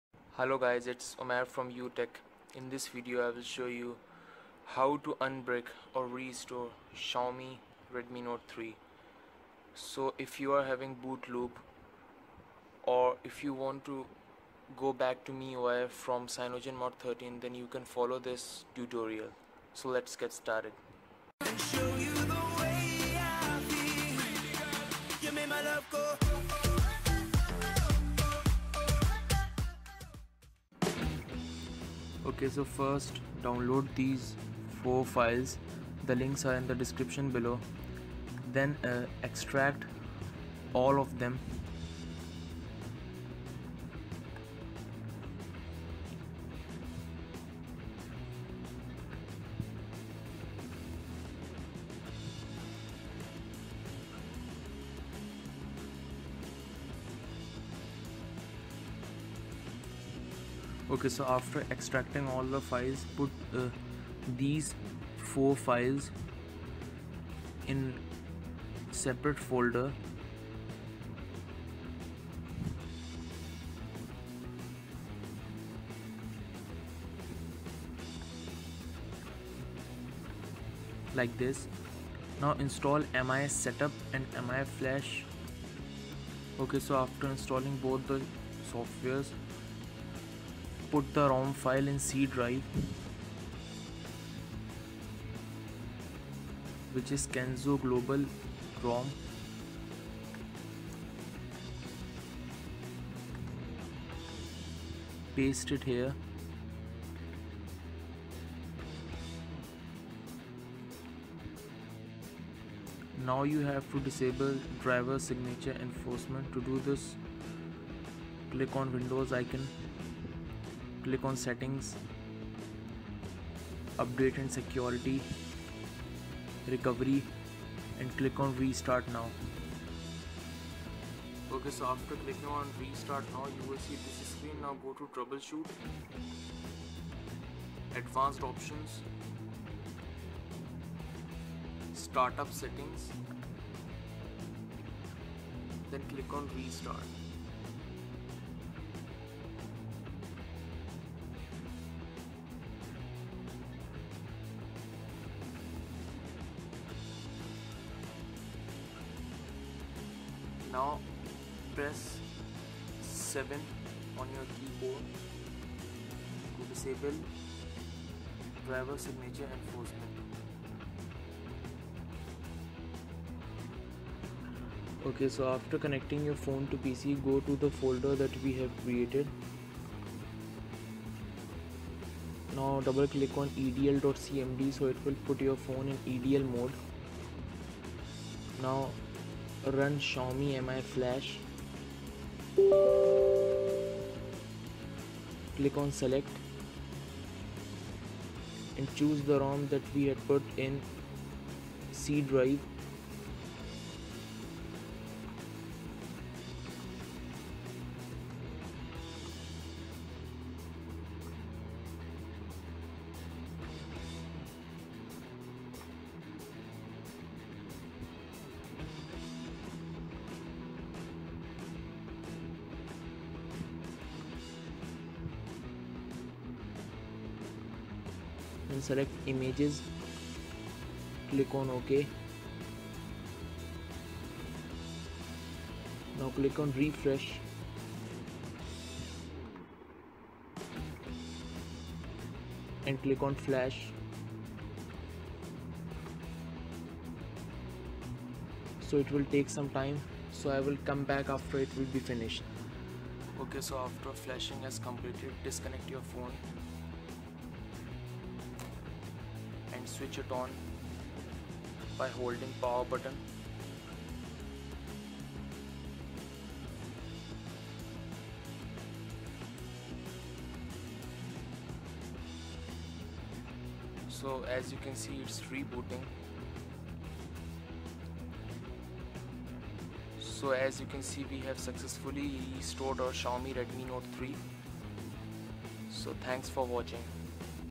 How to unbrick/restore xiaomi redmi note 3
In this video i will show you how to restore or unbrick redmi note 3.

U- tech